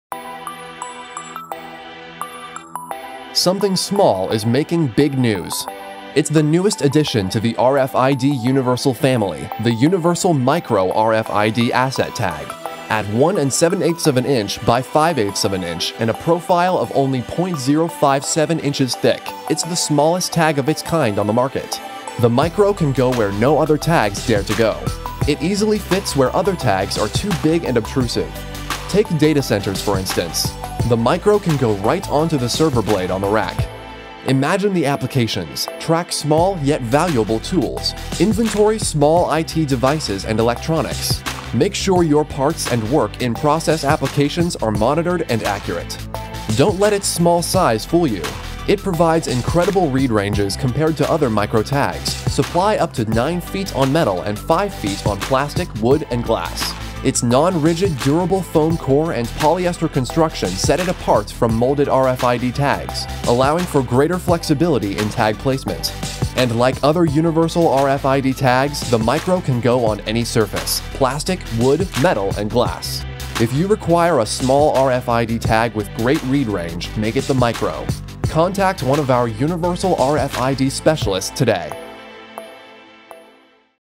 Universal Micro RFID Tag - Small RFID Tags With Great Read Range
The Universal Micro measuring at 1 7/8" x 5/8" is a great solution to your identification projects that require a small RFID tag with great read range. With a tiny footprint and low profile (57 mils.), the Universal Micro RFID Asset Tag easily fits where other tags are too big and obtrusive.

Supplying up to 9 ft. of read range on metal and 4-5 ft. on plastic, wood and glass, the Micro provides incredible read ranges compared to other tags in its class. Its non-rigid, durable, foam core and polyester construction sets the Micro apart from molded RFID tags allowing for greater flexibility in tag placement.